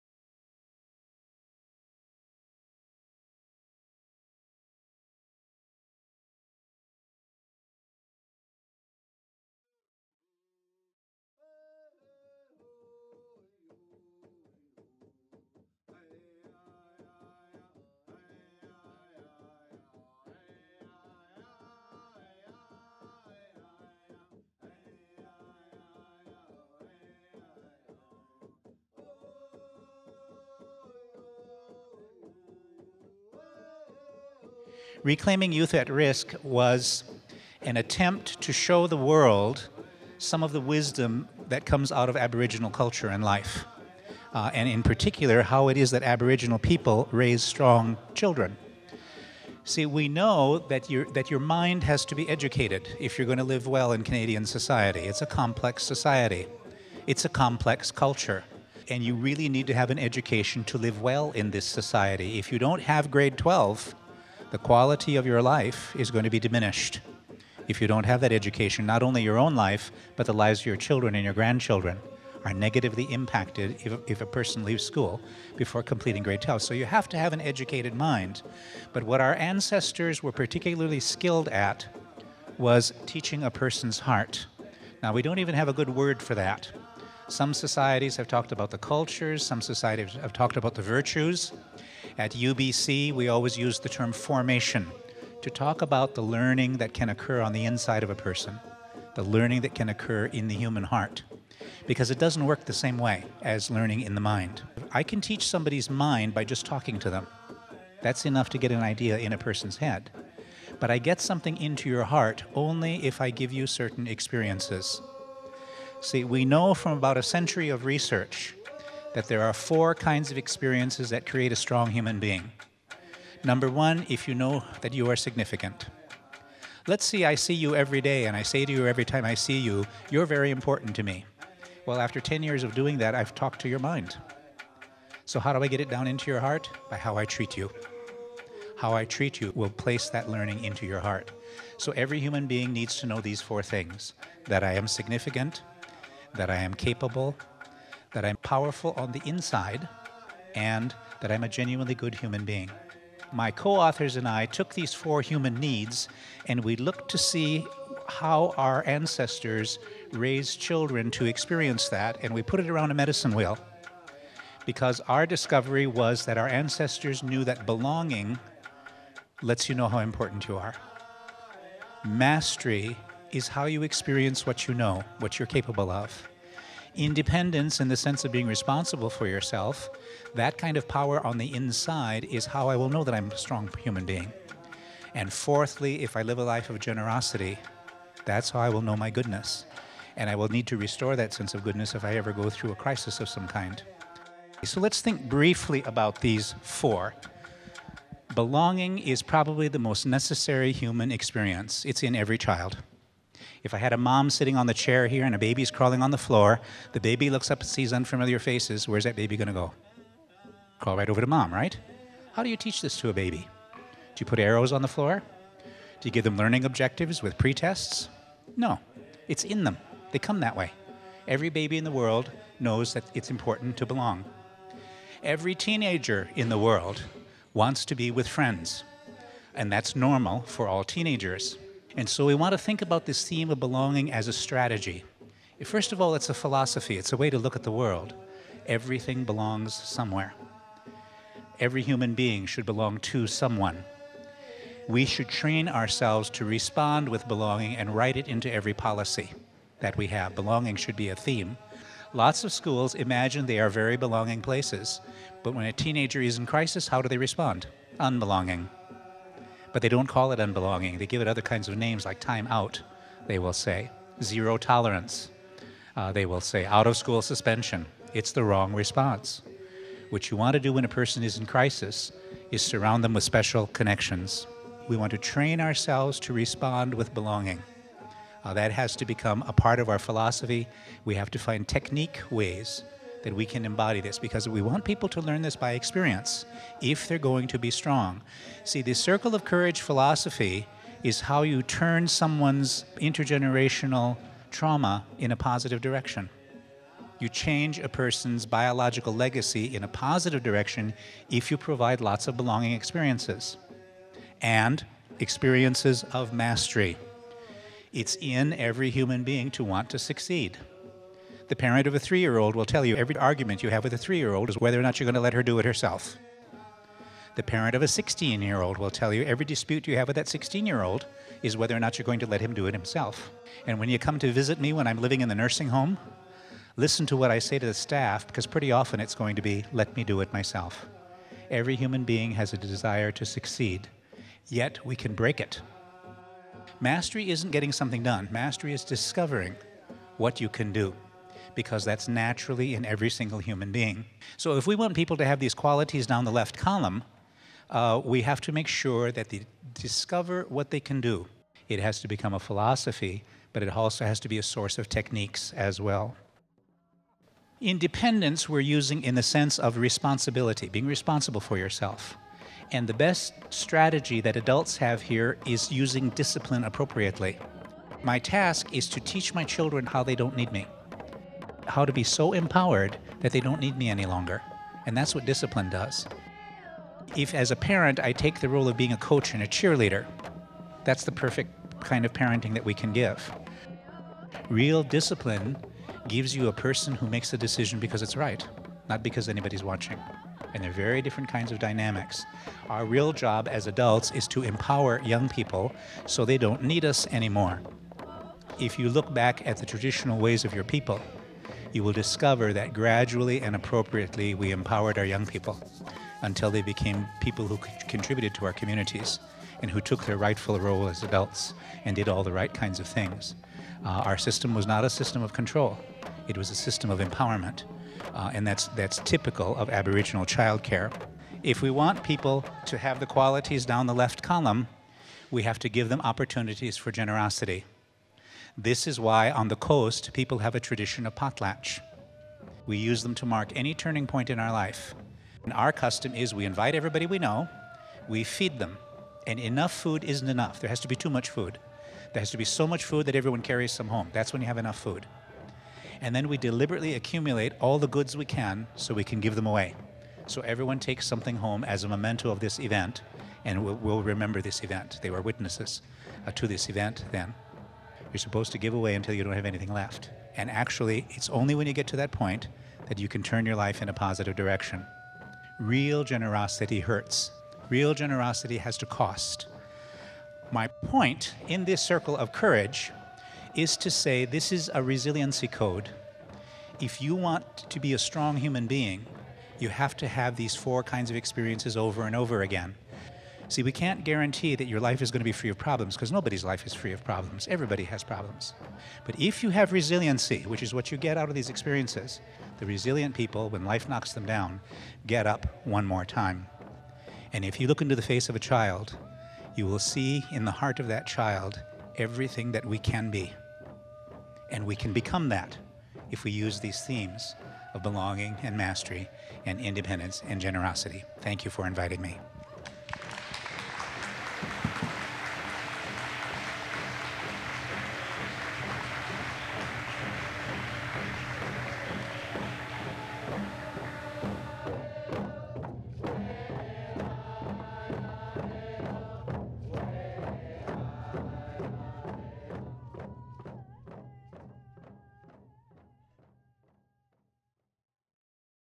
First Nations Principles of Learning
Used with permission from Martin Brokenleg.  . Introducing British Columbia First Nations Principles of Learning and the application of these concepts to Canadian education.
Information from BC First Nations Knowledge Keeper's Principles of Learning and Dr. Martin Brokenleg's presentation of the Circle of Courage at the Truth and Reconciliation Commission hearings held May 17 2013 in Williams Lake. Produced for School District 27 Cariboo-Chilcotin.
Audience: Educators, Students and general public. 9 minutes